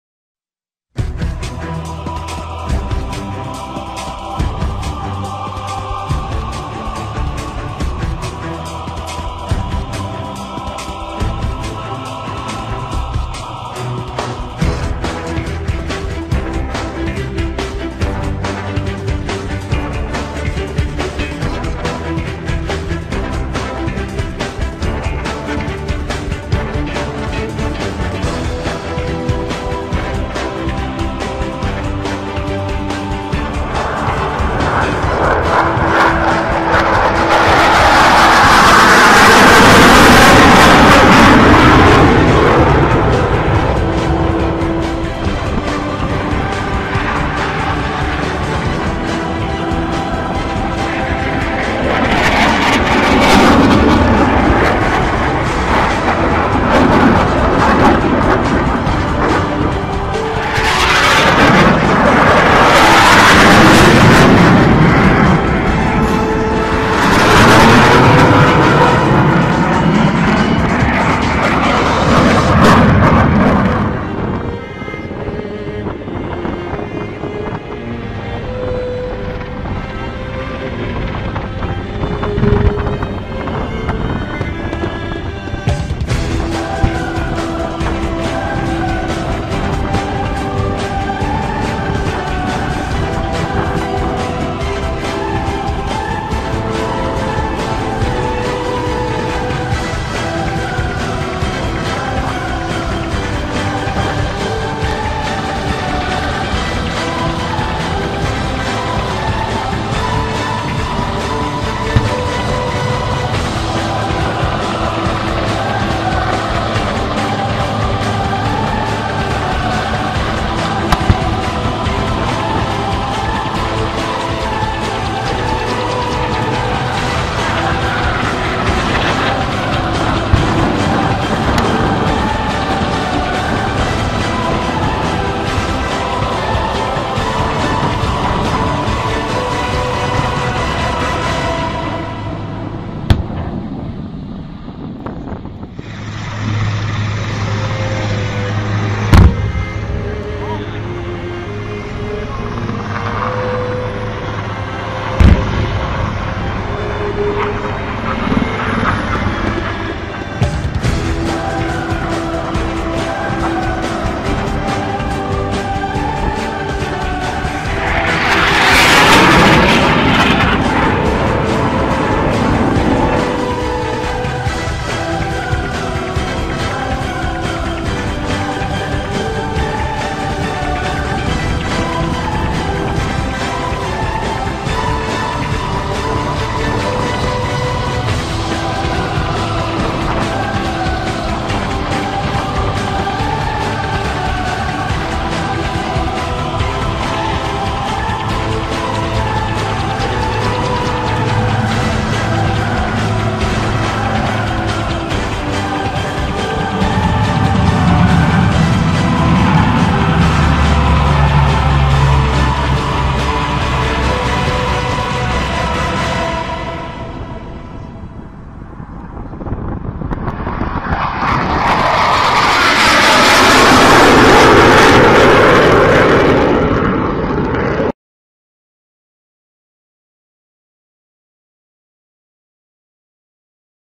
Waterkloof AAD 2016 movie
Excuse the footage shot it on my Cellphone, Sounds pretty good. Got to love Big Jets.